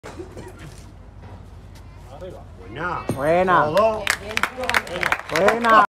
David Rodrigo de Barraires, 8 años, categoría Benjamin, barra de 2 kilos
Campeón Tiro
 Barra Aragonesa Miralbueno categoría benjamín